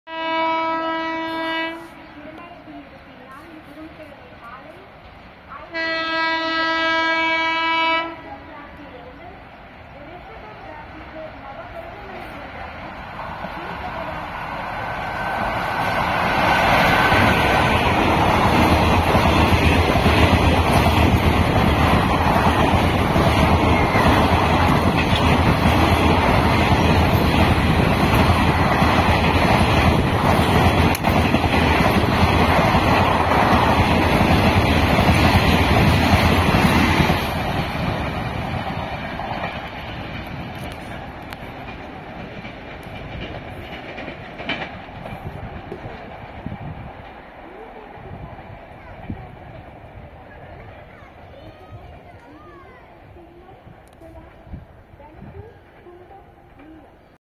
Mayiladuthurai Junction to Coimbatore Junction Jan Shatabdi Express
Jan Shatabdi Express skipping Lalapettai Railway station